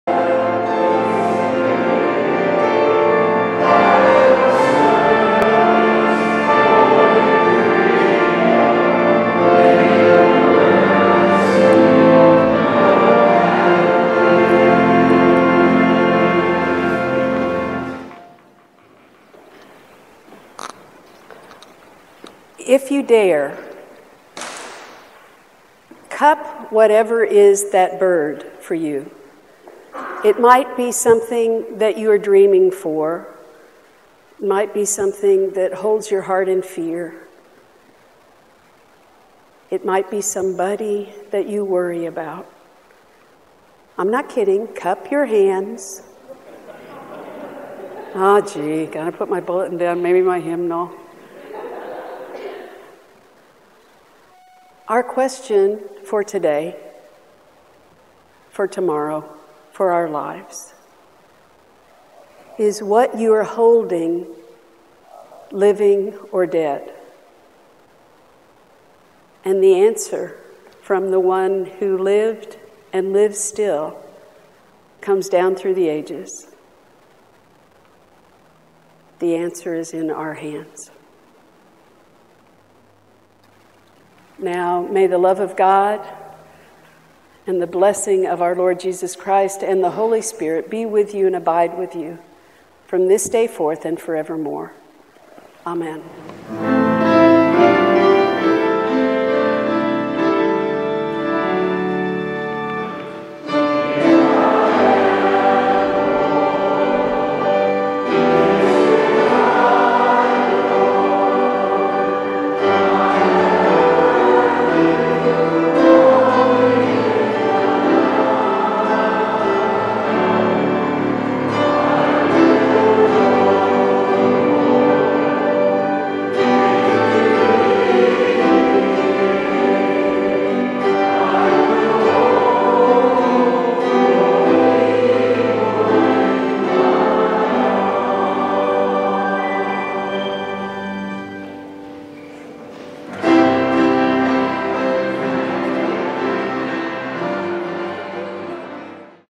The benediction from Rev. Lesley Brogan, July 9,2017
Rev. Lesley Brogan gives the benediction from her sermon "Comparing This Generation", Sunday, July 9, 2017